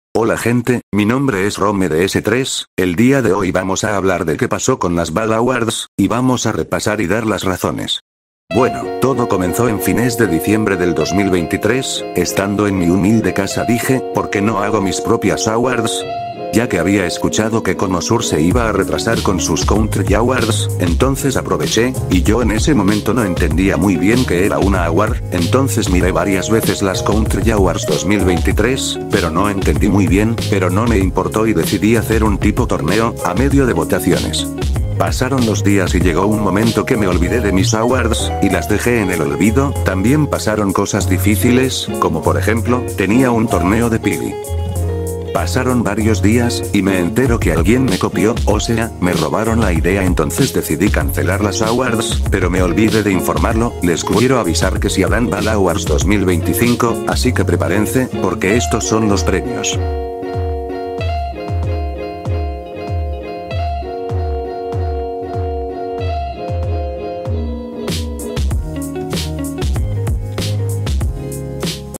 🍉🥕¿QUE PASÓ CON LAS BALL AWARDS 2024? 🍏🍋
HOY VOY A HABLAR DE QUE PASÓ CON MIS BALL AWARDS 2024
DARÉ RAZONES POR LAS CUALES YA NO EXISTEN
ESPERO QUE LAS SIGUIENTES BALL AWARDS 2025
TODO SALGA BIEN